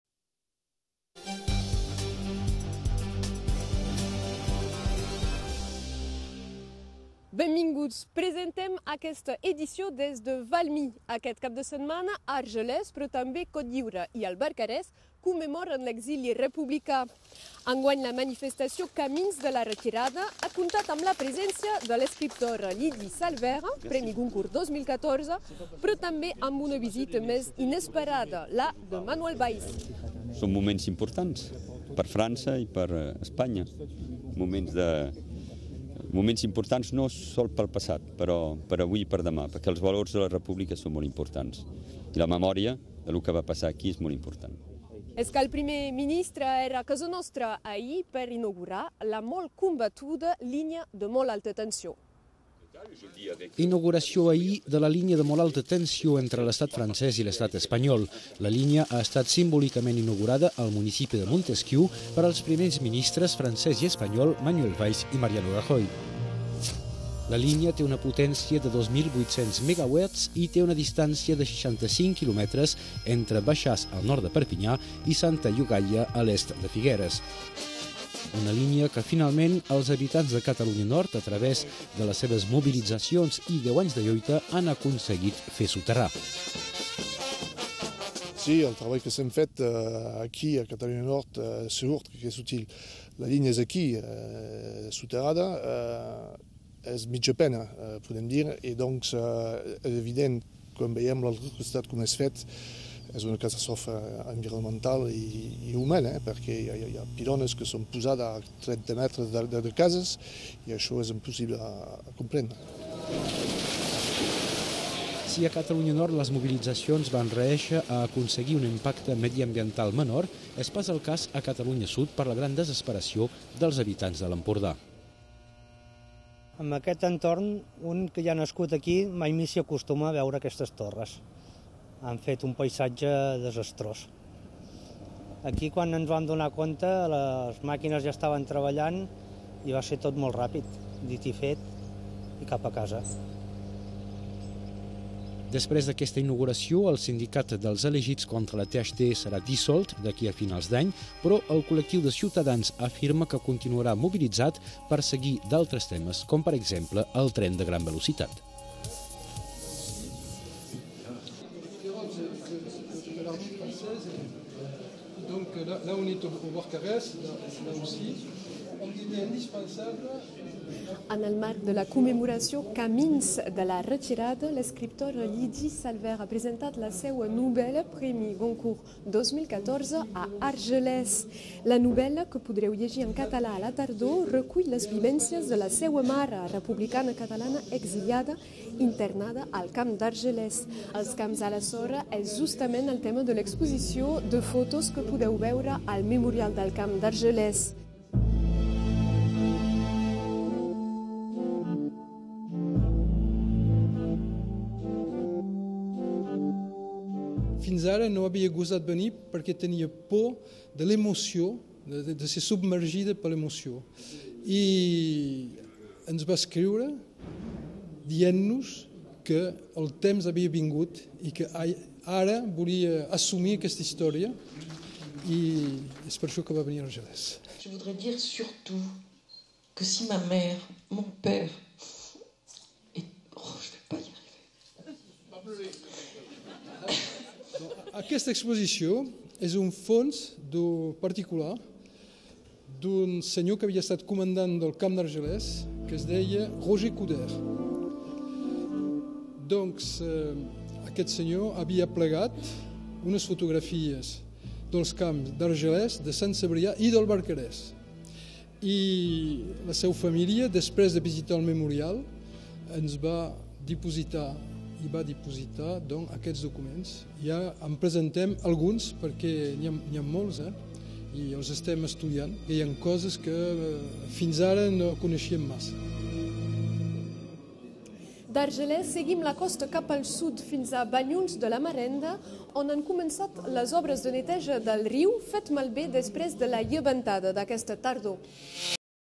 Manuel Valls en català a France 3 - 21/02/2015
Manuel Valls, primer ministre francès, en declaracions en català a France 3 - 21/02/2015